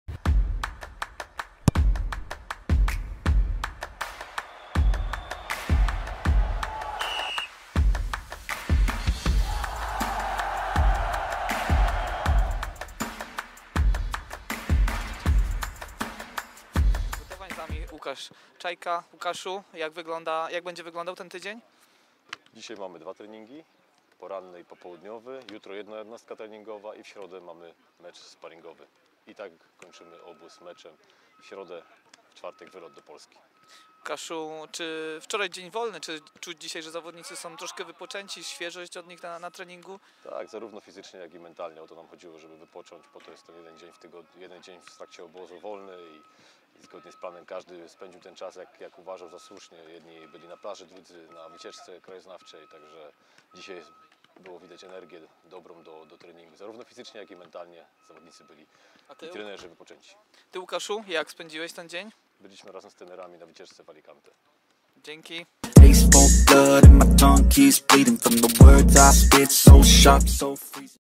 WYWIAD | Trener Łukasz Czajka
Przedstawiamy wywiad z trenerem Łukaszem Czajką.

Nasze biało-niebieskie produkty zakupisz online na oraz w sklepie FKS Stal Mielec przy ul. Solskiego 1.